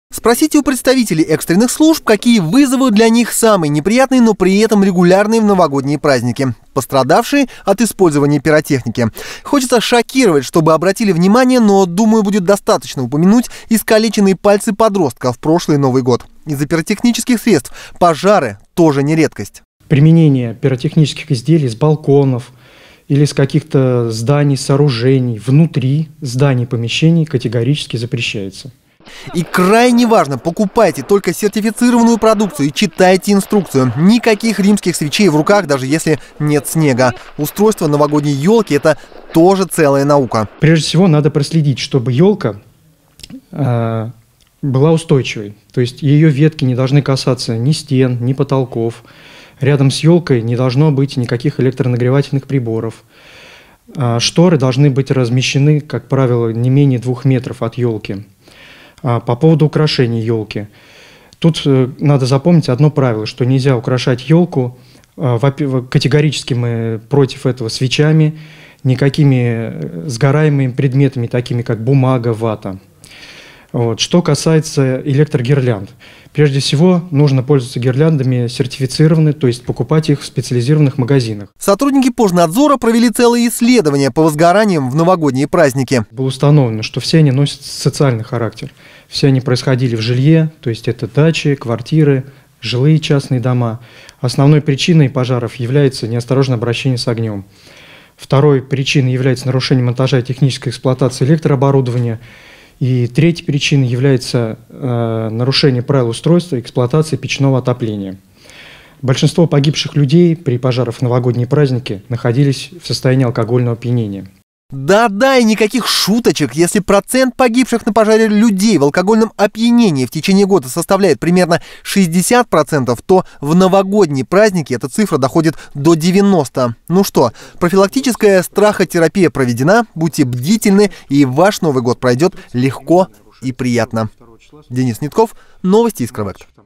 Пиротехника – плохая игрушка даже для взрослых. Правила использования!
«Маленький мальчик с петардой играл…» - так начинаются многие не смешные анекдоты. Чтобы не стать реальным воплощением анекдота с трагической развязкой, вам стоит услышать эти рекомендации по применению пиротехники в новогодние праздники.